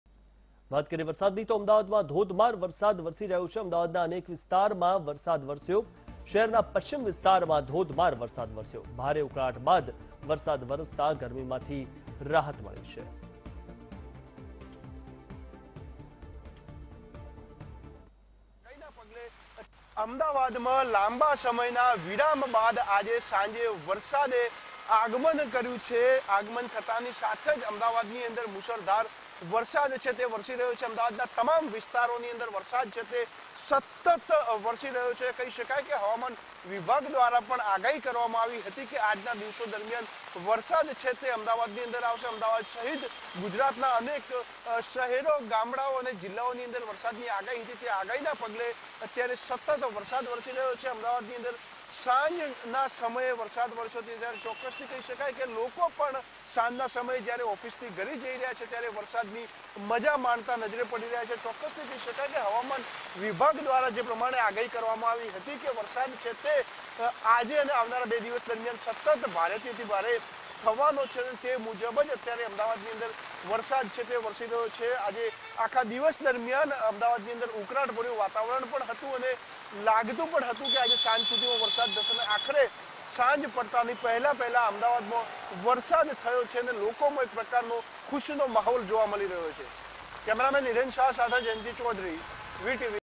Ahmedabad માં ભારે વરસાદ શરૂ | VTV Gujarati
Ahmedabad માં ભારે વરસાદ શરૂ : વસ્ત્રાપુર, બોડકદેવ, વેજલપુર, ગુરુકુળ સહિત પશ્ચિમ વિસ્તારોમાં મેઘરાજાની મહેર